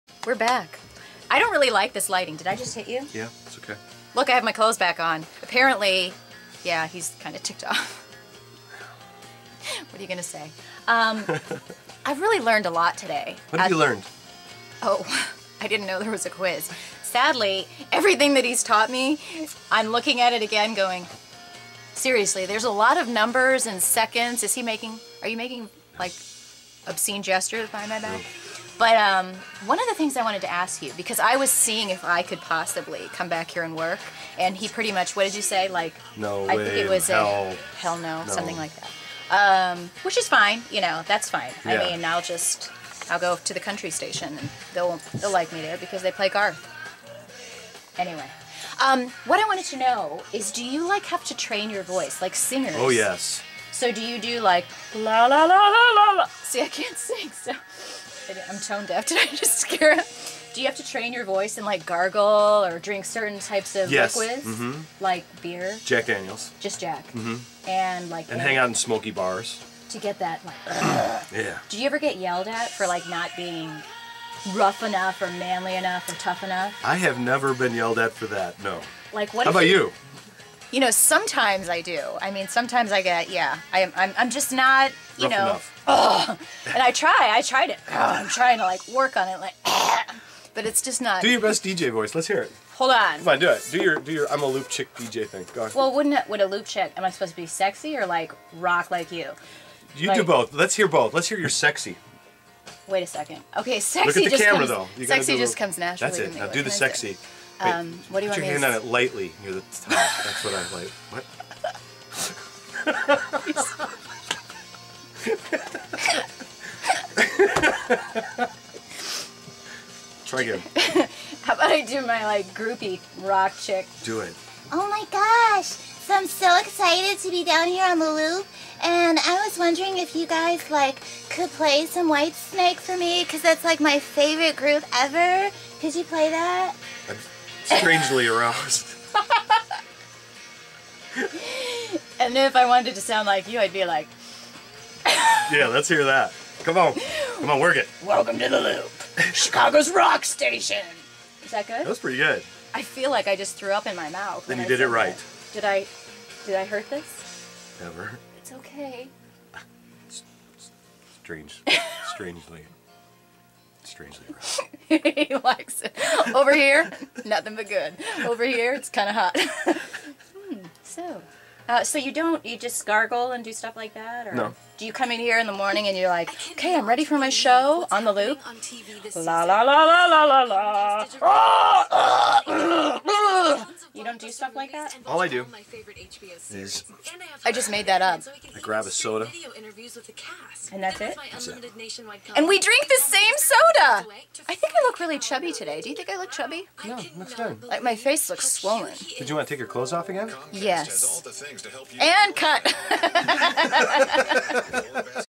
Loop 6
At Chicago's WLUP The Loop radio station with Pat Capone or @patrickcapone for Entertainment Mission with the FIESTAMOVEMENT